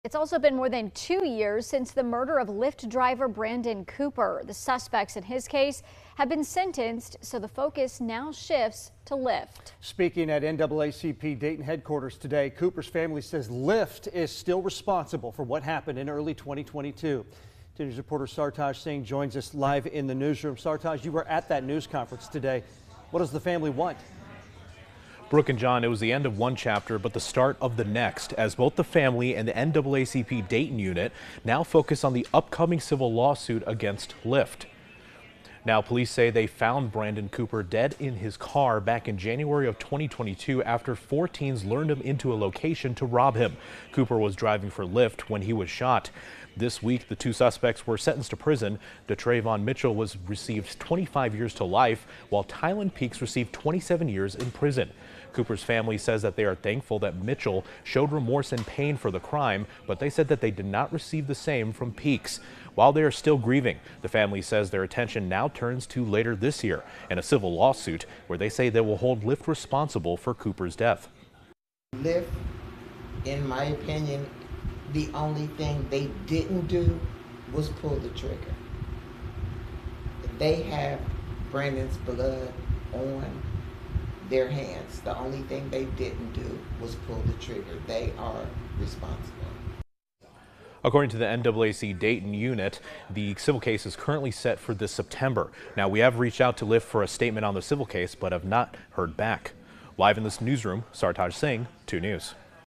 NAACP, family seeks accountability from Lyft after fatal shooting
It's been more than two years since Brandon Cooper's death, and while the suspects in his case have now been sentenced, the focus now shifts to Lyft.